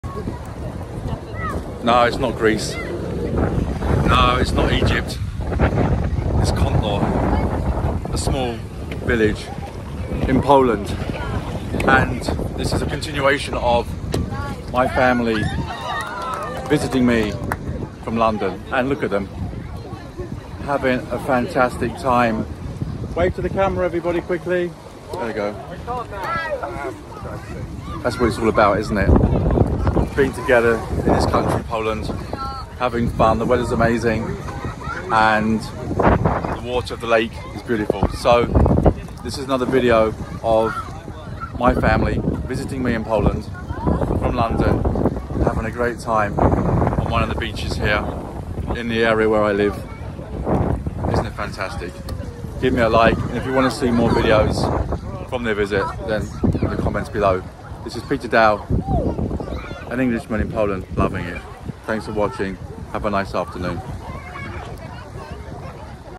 Family of Brits 🇬🇧in Poland 🇵🇱: We are lioving it !
Poland 🇵🇱: a brilliant place for a vacation.